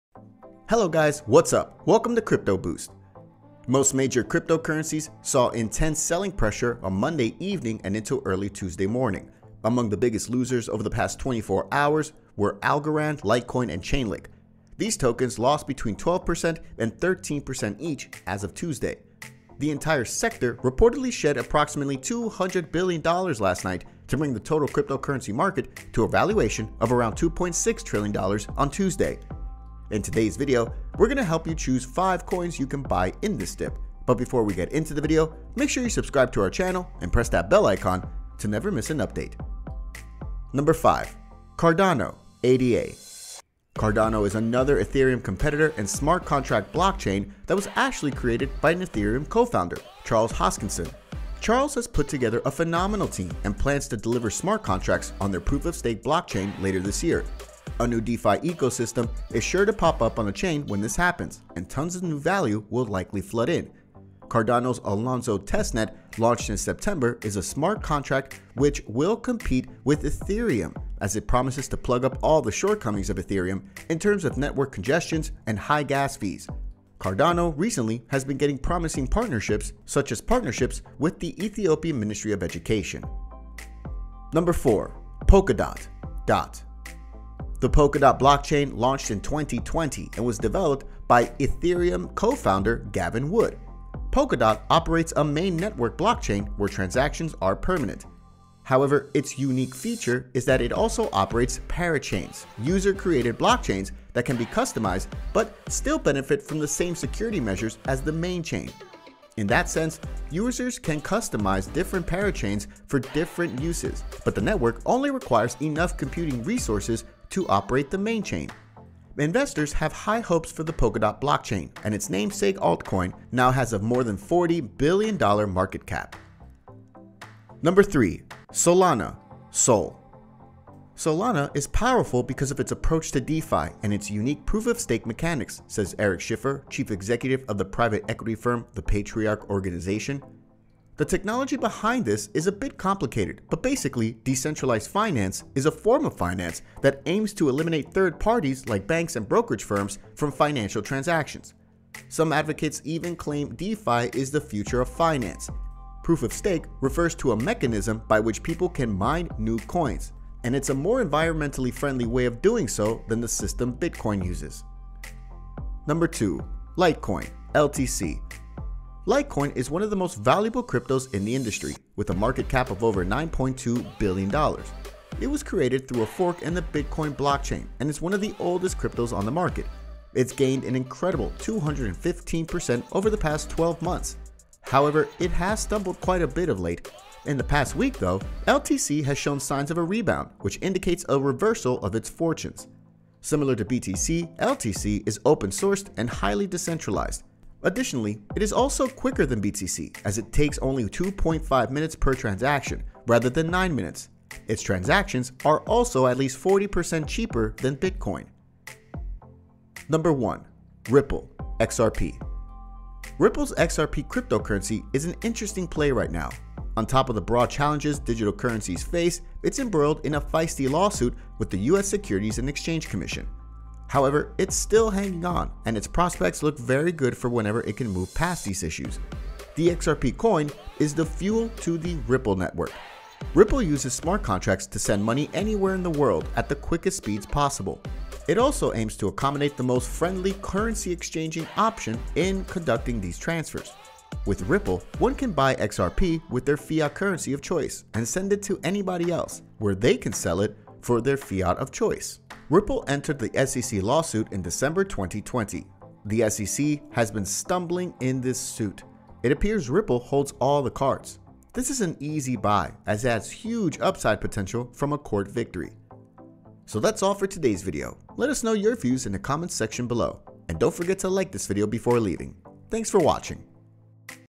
5 Coins You Can Buy in This DIP
5 Coins You Can Buy in This DIP

Hello guys whatsup , Welcome to Crypto Boost

Most major cryptocurrencies saw intense selling pressure Monday evening and into early Tuesday morning. Among the biggest losers over the past 24 hours were Algorand, Litecoin and Chainlink. These tokens lost between 12% and 13% each, as of Tuesday.

The entire sector reportedly shed approximately $200 billion last night, to bring the total cryptocurrency market to a valuation of around $2.6 trillion on Tuesday. In today’s video we’re going to help you choose 5 coins you can buy in this dip.

5. Cardano (ADA)
Cardano is another Ethereum competitor and smart contract blockchain that was actually created by an Ethereum co-founder, Charles Hoskinson. Charles has put together a phenomenal team and plans to deliver smart contracts on their proof of stake blockchain later this year. A new DeFi ecosystem is sure to pop up on the chain when this happens, and tons of new value will likely flood in.

Cardano’s Alonzo Testnet, launched in September is a smart contract which will compete with Ethereum as it promises to plug up all the shortcomings of Ethereum in terms of network congestions and high gas fees. Cardano, in recent times, has been getting promising partnerships such as the partnership with the Ethiopian Ministry of Education.